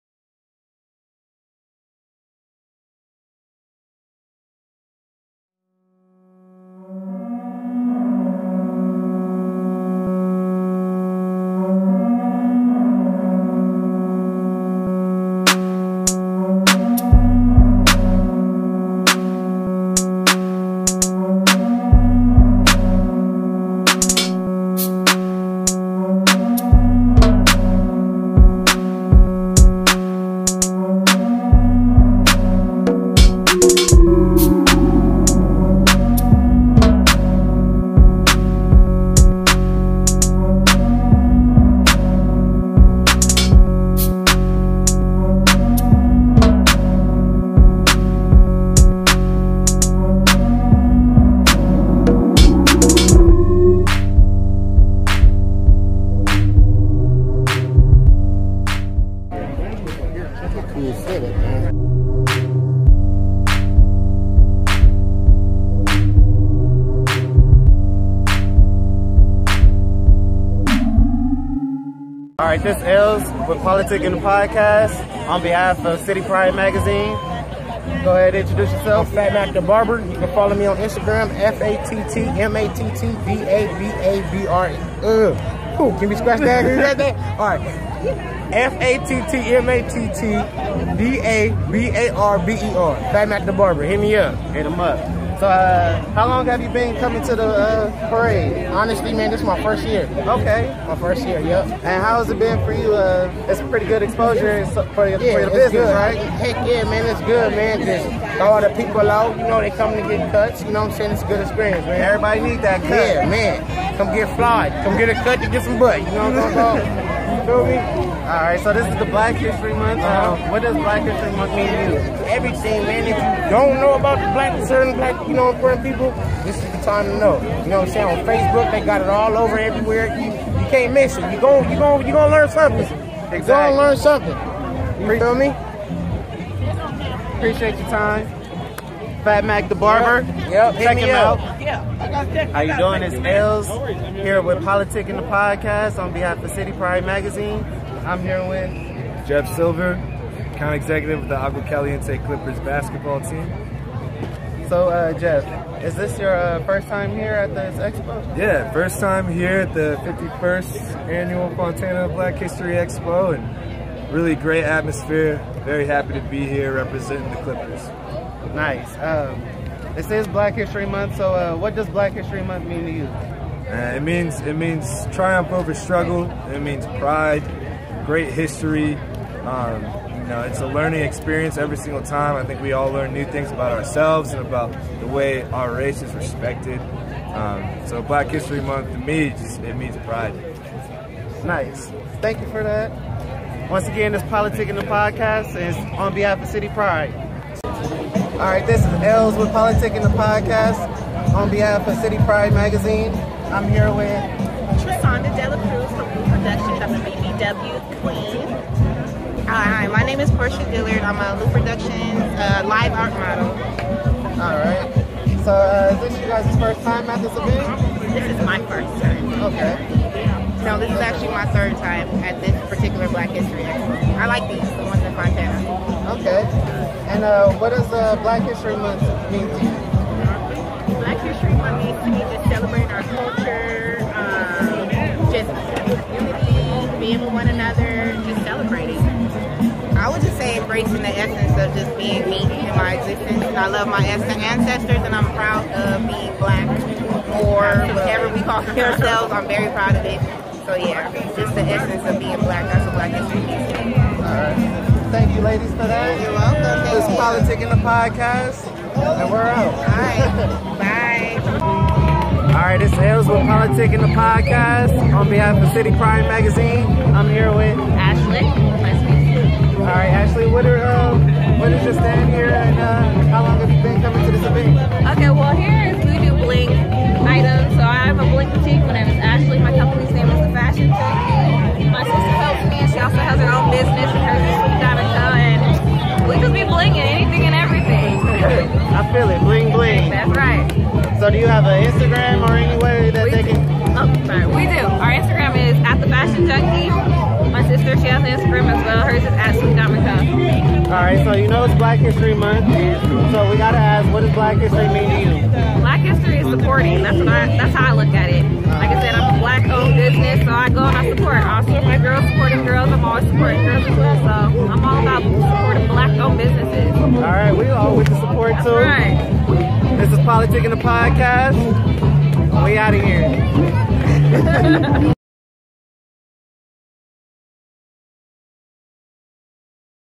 Black History Parade and Expo in Fontana Ca
PolitiKan The Podcasts on be half of City Pride Magazine at the Fontana Black History Parade.